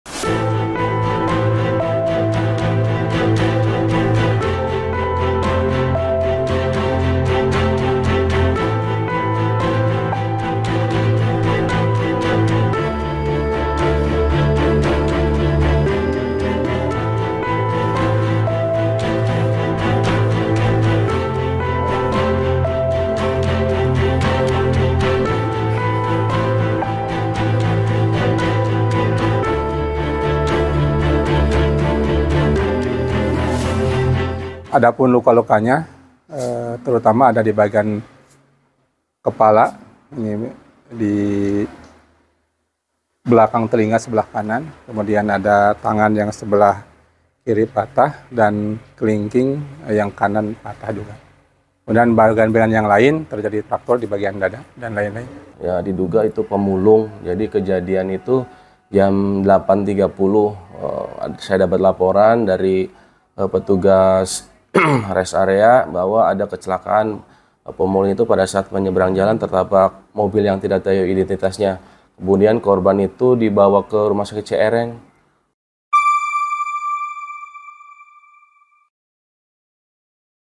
Pria Pencari Kayu Bakar Tewas di Parit Tol Cipali, Diduga Korban Tabrak Lari
Seorang pencari kayu bakar ditemukan tewas di parit Tol Cipali, Subang, Jabar. Diduga pencari kayu bakar itu menjadi korban tabrak lari kendaraan di tol itu. Petugas tol mengevakuasi korban dari KM 86 arah Palimanan ke Jakarta.

Android: inews.id/android
iPhone: inews.id/ios